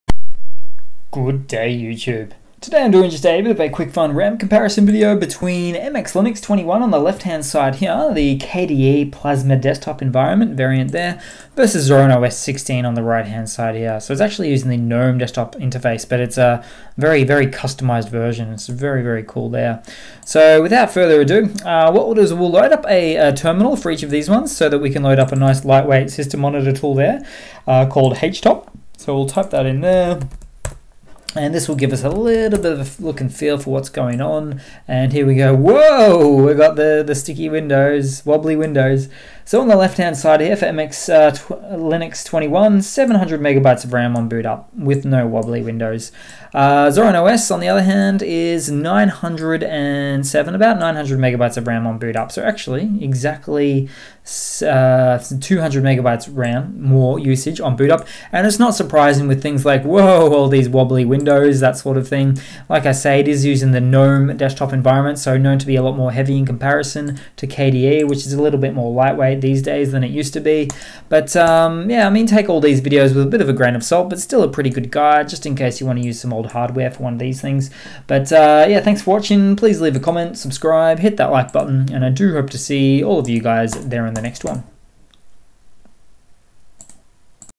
MX Linux 21 (KDE) vs Zorin OS 16: RAM Usage
Let's give these two a red-hot-go!